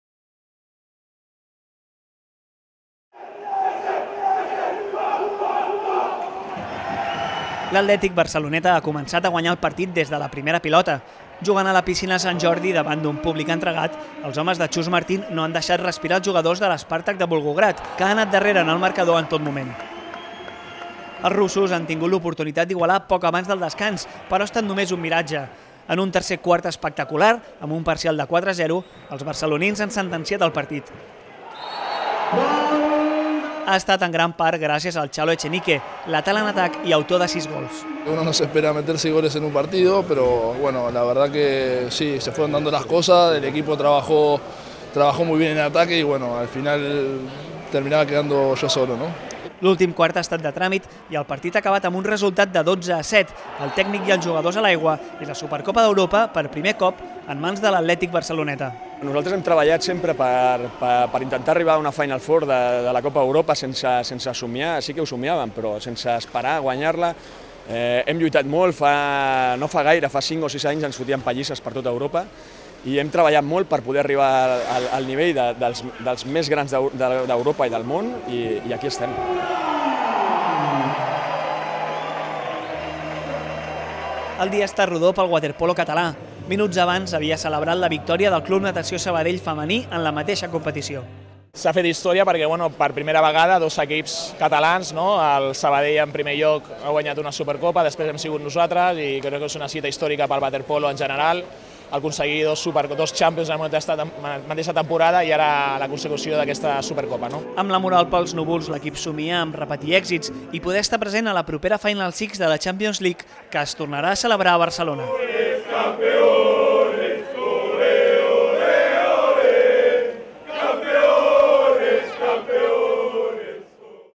L’Atlètic-Barceloneta aixeca la Supercopa d’Europa de Waterpolo
El Club Natació Atlètic-Barceloneta s’ha endut un nou títol continental. Els mariners han aixecat la Supercopa d’Europa després de derrotar al Spartak de Volgograd rus (12-7) a la piscina Sant Jordi de Barcelona. Més informació